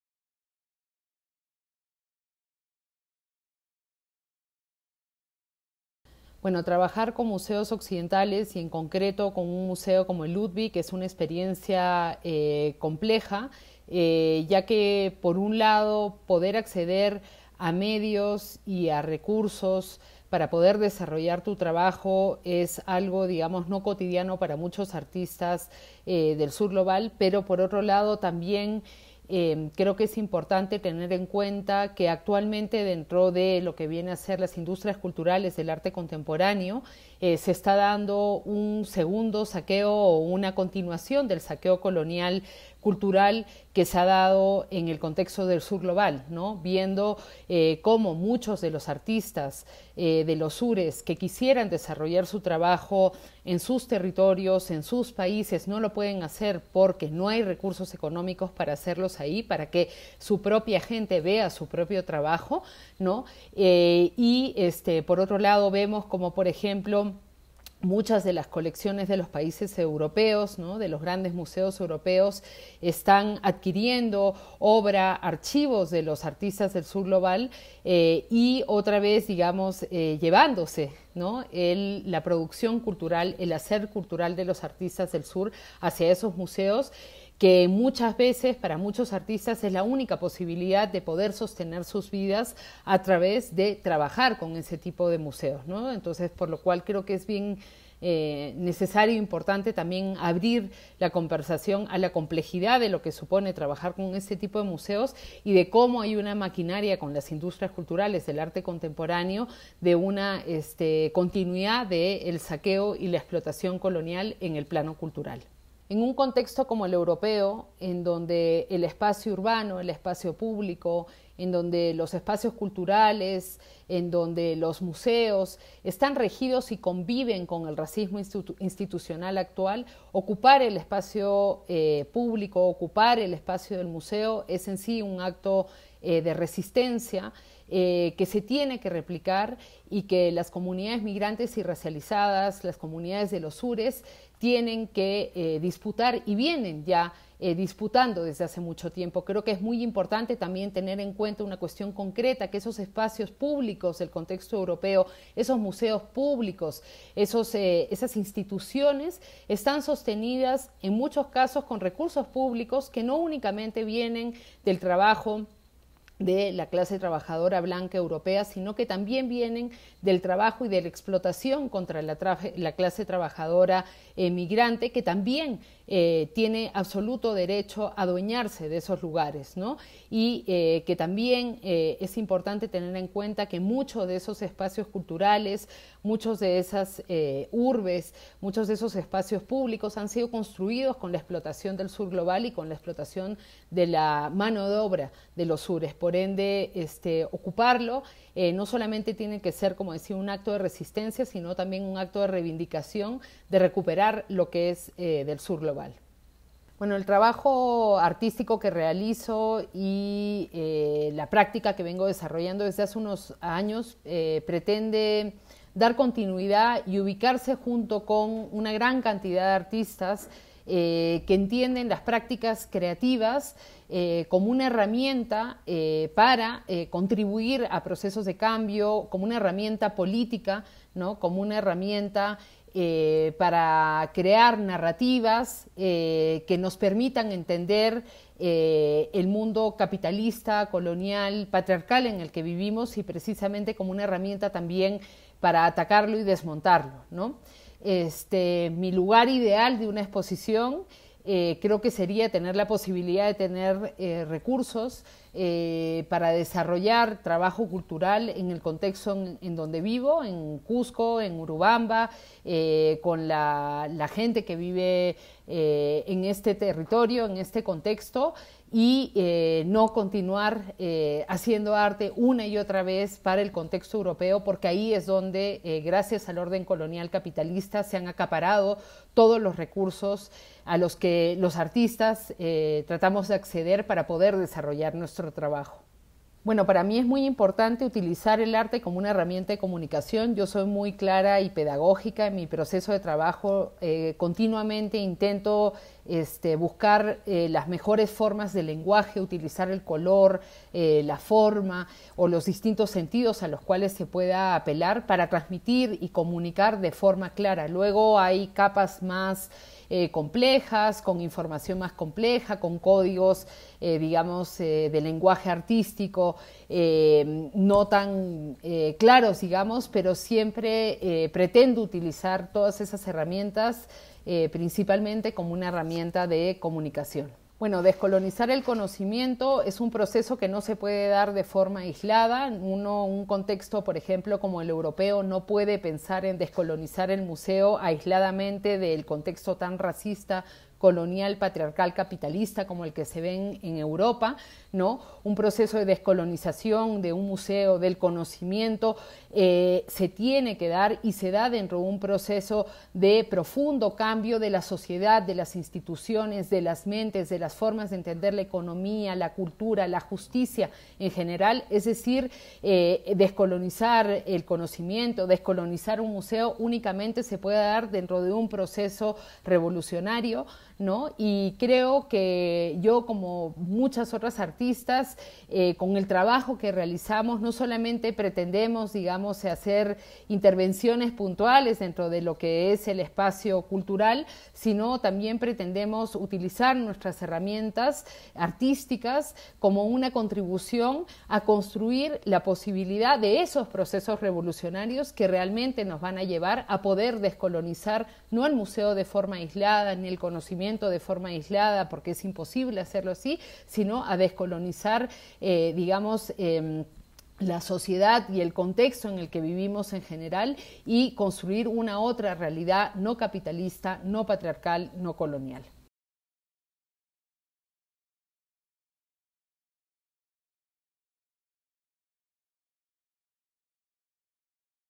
Anticolonial Interventions Revisited with Daniela Ortiz
As part of the Decolonial Studies Program (DSP), an exhibition seminar dealt with the exhibition HERE AND NOW. Anti-colonial Interventions at Museum Ludwig (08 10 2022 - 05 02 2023), which opened up other perspectives of knowledge on the permanent collection of the museum. The four artists Paula Baeza Pailamilla (b. 1988 in Chile), Daniela Ortiz (b. 1985 in Peru), Pável Aguilar (b. 1989 in Honduras) and Paloma Ayala (b. 1980 in Mexico) presented their artistic practice, talked about their position as artists from Latin America, as well as their interventions within the exhibition. For our four-part video series, the artists climbed onto the rooftop of the Museum Ludwig and reflected in performances and interviews on their exhibition works and their artistic practice within a Western museum.

Daniela Ortiz lives and works in Peru and Spain. The artist is committed to combating racism, colonial and patriarchal structures and creates new visual narratives through the means of painting, collage, performances and in the form of children's books. In the exhibition, she commented on Max Ernst's Die Jungfrau züchtigt das Jesuskind vor drei Zeugen (The Virgin Chastises the Child Jesus in Front of Three Witnesses, 1921) and criticized state-run shelters under whose supervision refugee children died while the testimony of white employees counted for more than the information of racialized mothers.